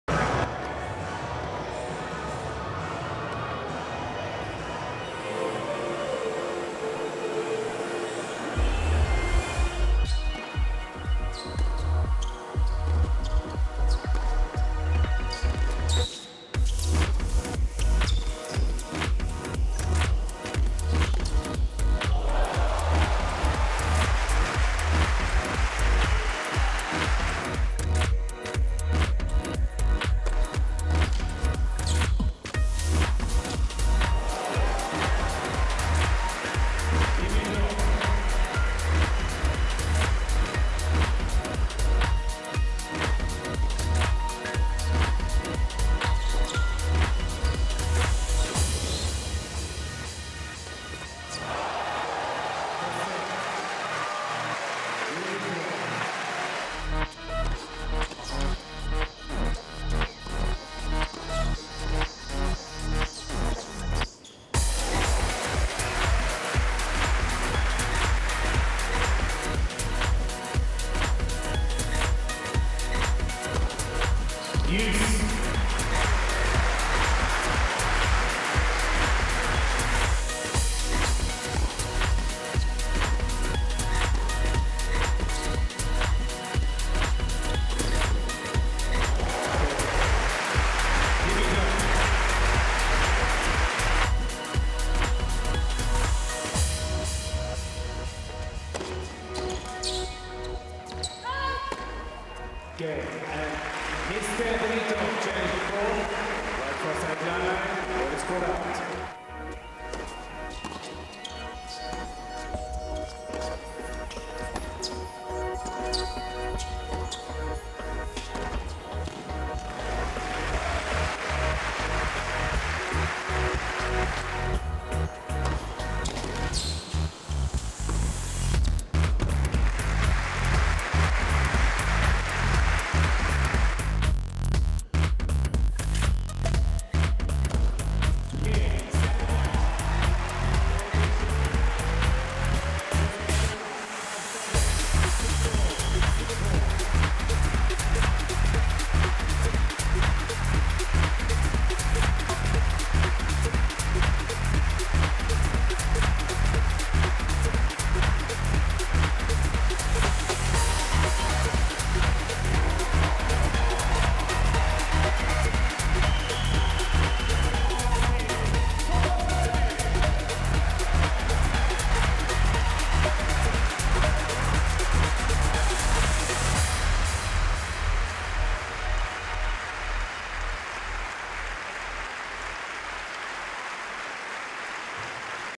Stockholm 2014 Final Highlights Berdych Dimitrov
Watch highlights of the 2014 If Stockholm Open final, featuring Tomas Berdych and Grigor Dimitrov.